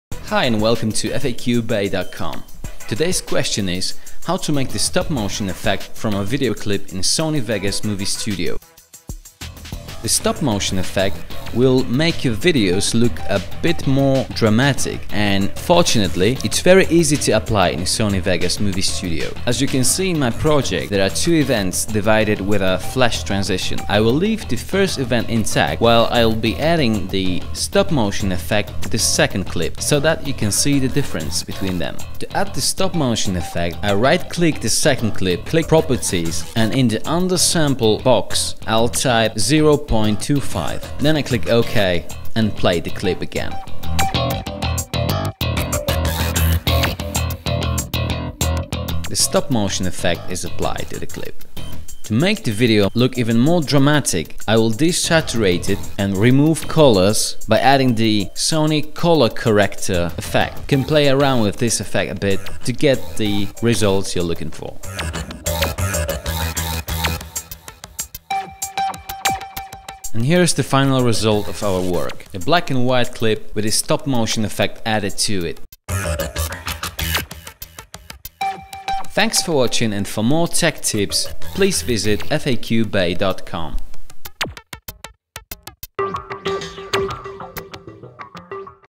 How to Make Stop Motion Effect in Sony Vegas Movie Studio
This tutorial explains how to make the stop-motion effect from an animated video clip in Sony Vegas and Sony Vegas Movie Studio.